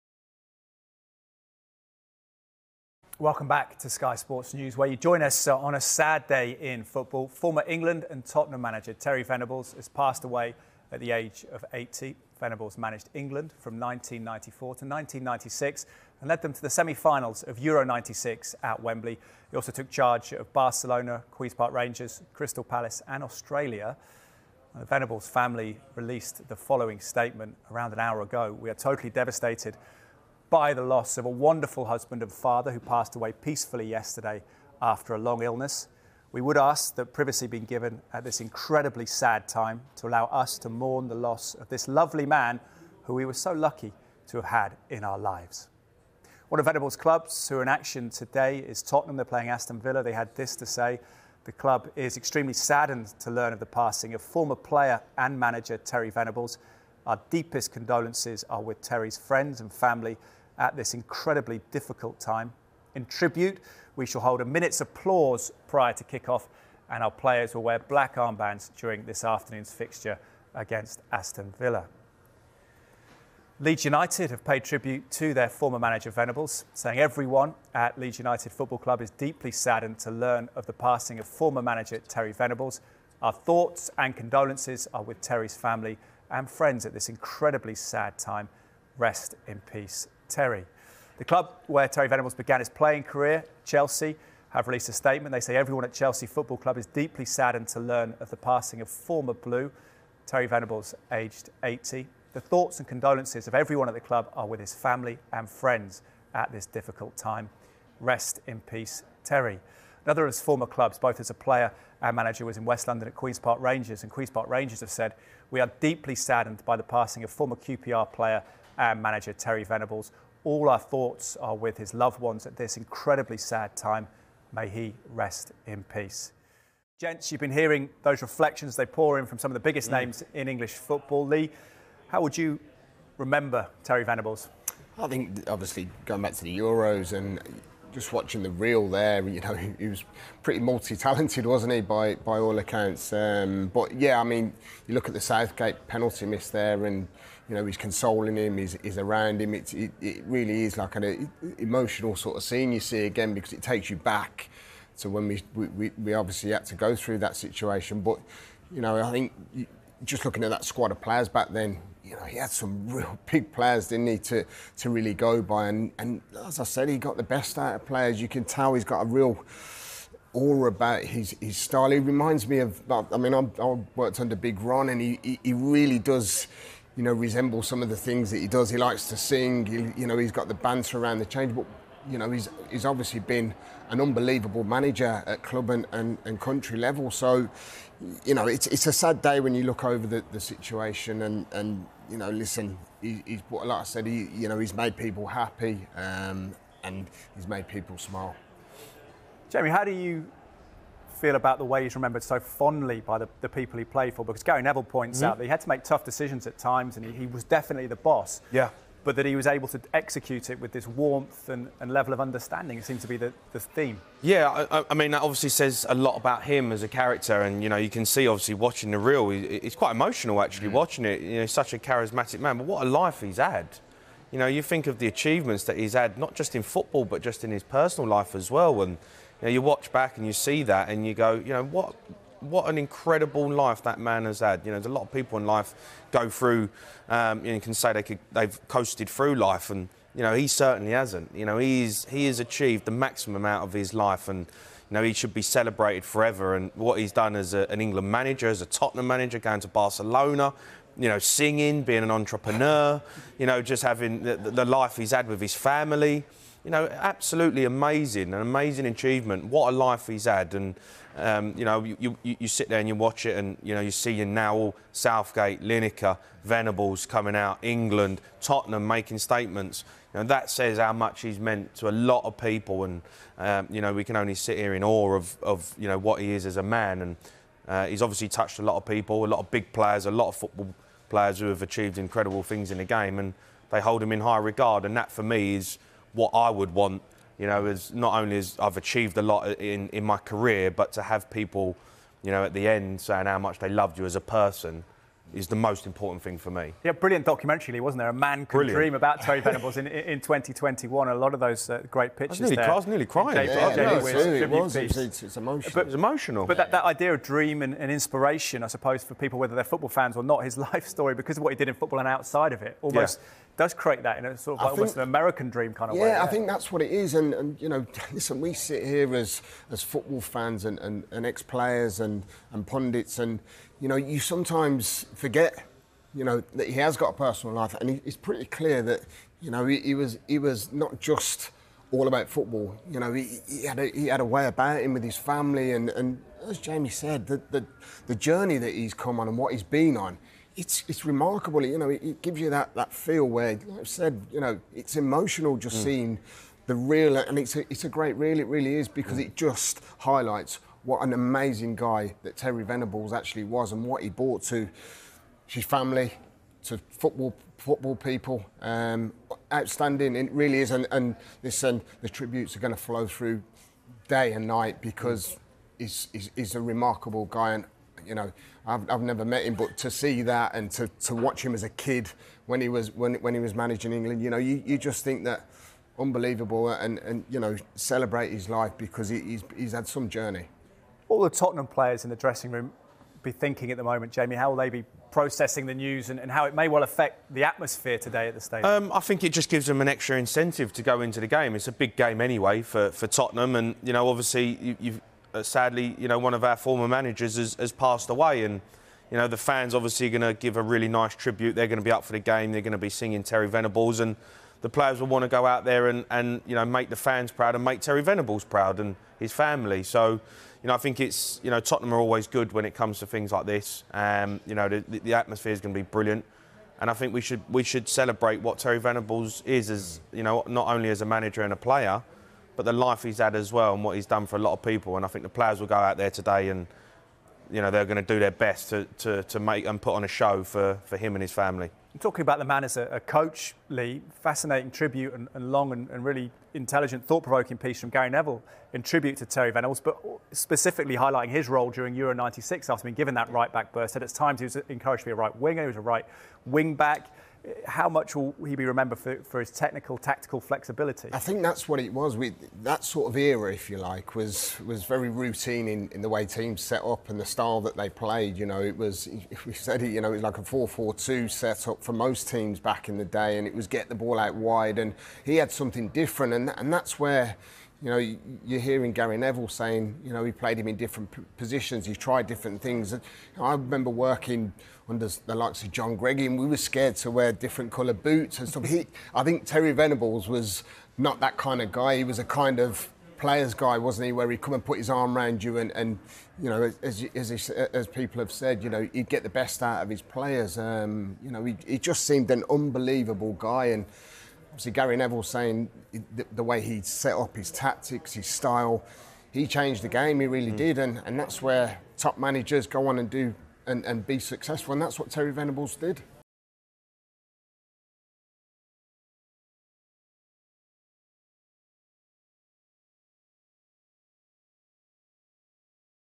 Tributes paid to former England and Tottenham manager Terry Venables
Former England manager Terry Venables has died aged 80.

Venables at the helm for England's unforgettable run to the semi-finals of Euro 96 after forging a highly successful club managerial career with Crystal Palace, QPR, Barcelona and Tottenham.

He also made over 500 appearances for Chelsea, Tottenham, QPR and Crystal Palace as a player.

A statement from the family of Venables said: "We are totally devastated by the loss of a wonderful husband and father who passed away peacefully yesterday after a long illness.

"We would ask that privacy be given at this incredibly sad time to allow us to mourn the loss of this lovely man who we were so lucky to have had in our lives."